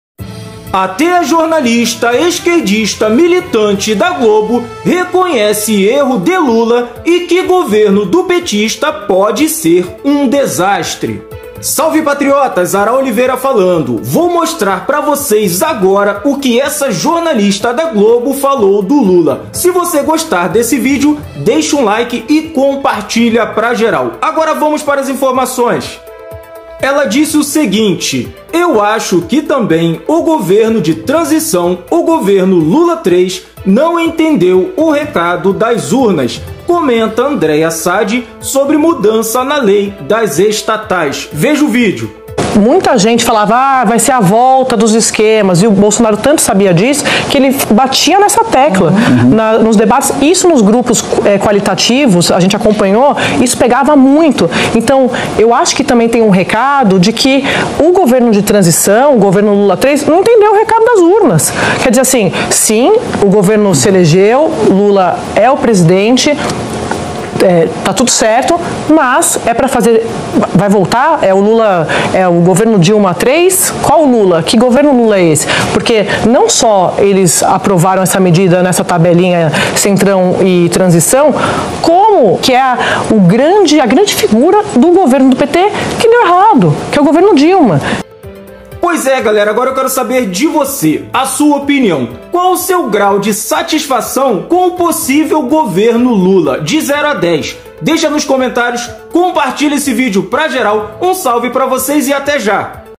ANDRÉIA SADI DIZ QUE GOVERNO DE LULA PODE SER UM DESASTRE...
ATÉ A LACRADORA MILITANTE DA GLOBO, ANDRÉIA SADI, DIZ QUE LULA ERROU E QUE GOVERNO DO PETISTA PODE SER UM DESASTRE...